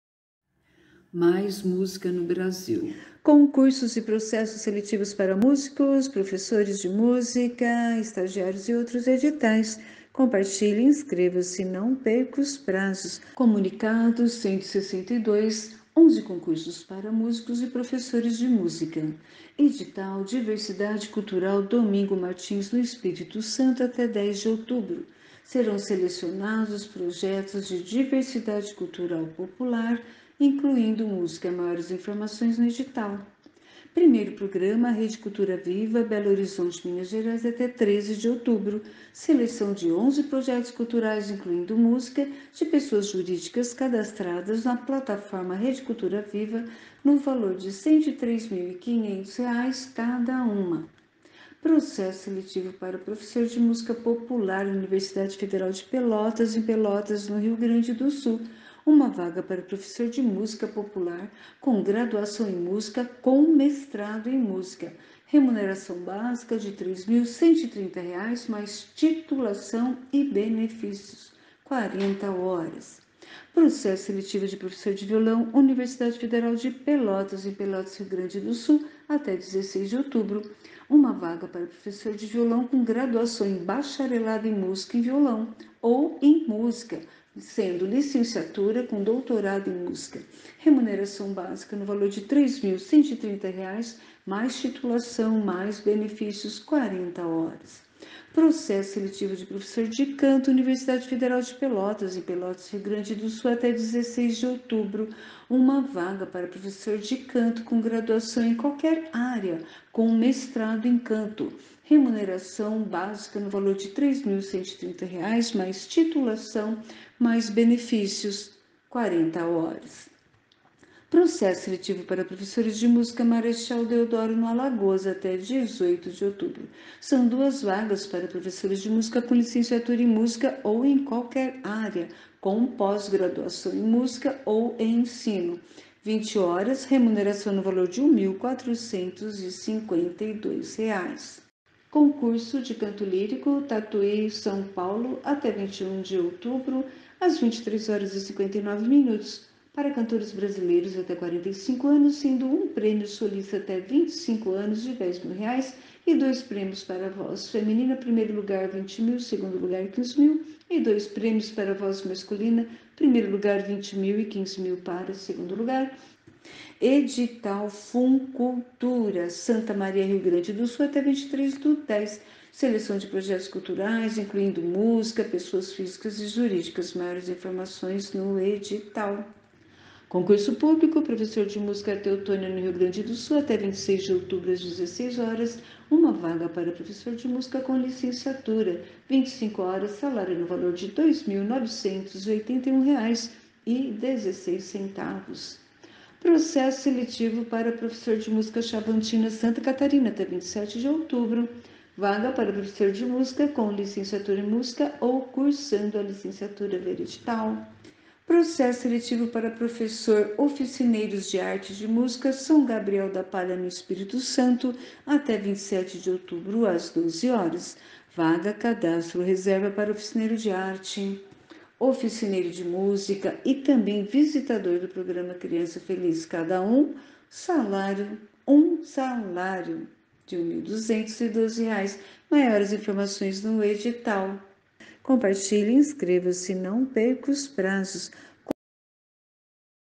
0162 MAIS MÚSICA NO BRASIL: 11 Concursos para MÚSICOS, Professores de MÚSICA, Editais 2022-23
COMUNICADO 162:  11 CONCURSOS P MÚSICOS E PROFESSORES DE MÚSICA
CURTE E COMPARTILHE

162.1 AVISO 7053: EDITAL DIVERSIDADE CULTURAL – DOMINGO MARTINS/ES
DATA: até 10/10
RESUMO: seleção projetos diversidade cultural POPULAR, incluindo Música

162.2 AVISO 7054: 1º PROGRAMA REDE CULTURA VIVA – BELO HORIZONTE/MG
DATA: até 13/10
RESUMO: seleção 11 projetos culturais/música, pessoas jurídicas cadastrados na Plataforma Rede Cultura Viva, valor R$ 103 mil cada

162.3 AVISO 7047: PROC SELET PROF MÚSICA POPULAR – UFPEL – PELOTAS/RS
DATA: até 16/10
RESUMO: 1 vaga prof música popular, Graduação em Música COM Mestrado em Música, remuneração básica R$ 3.130,85 + titulação e benefícios (40h/a)

162.4 AVISO 7048: PROC SELET PROF VIOLÃO – UFPEL – PELOTAS/RS
DATA: até 16/10
RESUMO: 1 vaga prof Violão, Graduação em Bacharelado em Música - Violão OU Música - Licenciatura COM Doutorado em Música, remuneração básica R$ 3.130,85 + titulação e benefícios (40h/a)

162.5 AVISO 7049: PROC SELET PROF CANTO – UFPEL – PELOTAS/RS
DATA: até 16/10
RESUMO: 1 vaga prof Canto, Graduação em qualquer Área COM Mestrado em Canto,  remuneração básica R$ 3.130,85 + titulação e benefícios (40h/a)
162.6 AVISO 7050: PROC SELET PROFS MÚSICA – MARECHAL DEODORO/AL
DATA: até 18/10
RESUMO: 2 vagas profs Música, licenciatura Música ou em qq área e pós graduação em música ou em ensino, R$ 1.452,00 (20h/a)

162.7 AVISO 7035: CONCURSO DE CANTO LÍRICO – TATUÌ/SP
DATA: até 21/10, às 23h59min
RESUMO: para cantores brasileiros até 45 anos, sendo 1 prêmio solista até 25 anos (R$10 mil), e 2 prêmios voz feminina (R$20mil e R$15mil) e 2 prêmios voz masculina (R$20mil e R$15 mil) até 45 anos

162.8 AVISO 7055: EDITAL FUNCULTURA SANTA MARIA/RS
DATA: até 23/10
RESUMO: seleção de projetos culturais, incluindo MÚSICA, pessoas físicas e jurídicas

162.9 AVISO 7030: CONC PÚBLICO PROF MÚSICA – TEUTÔNIA/RS
DATA: até 26/10, às 18h
RESUMO: 1 vaga prof música Infantil, 25 h/R$ 2.981,16

162.10 AVISO 7036: PROC SELET PROF MÚSICA – XAVANTINA/SC
DATA: até 27/10
RESUMO: vaga prof música, licenciatura música, Ou cursando a licenciatura

162.11 AVISO 7044: PROC SELET PROF OFICINEIROS DE ARTE E DE MÚSICA – SÃO GABRIEL DA PALHA/ES
DATA: até 27/10, às 12h
RESUMO: vaga CR para oficineiro de arte; oficineiro de música; visitador do Programa Criança Feliz, 1 salário
INSCR: presencial, na Secretaria Municipal de Assistência Social, Rua 14 de Maio, nº 54, Bairro Glória